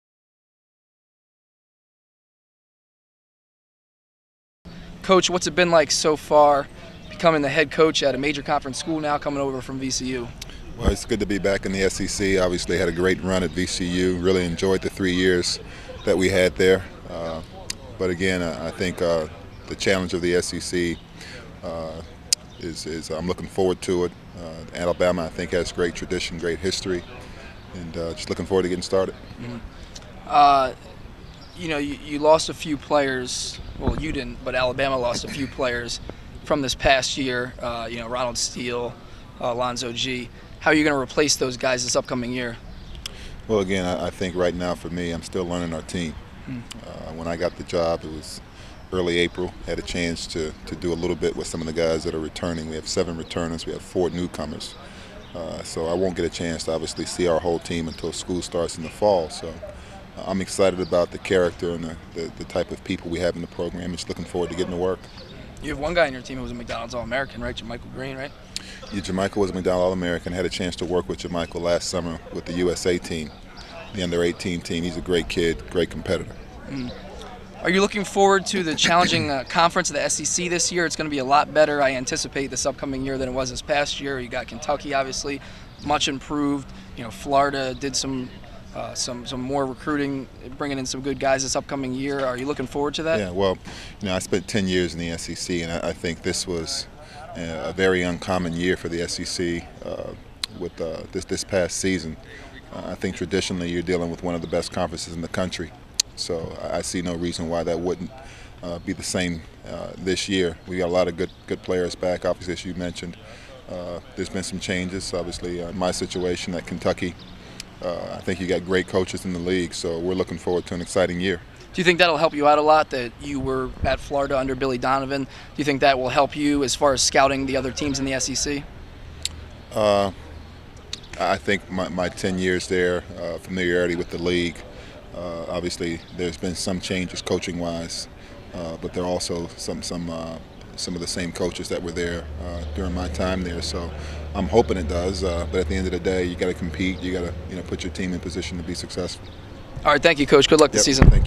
Interview with Alabama Head Coach Anthony Grant
Alabama head coach Anthony Grant is interviewed by The Hoops Report at the Reebok All-American Camp in Philadelphia on July 9, 2009.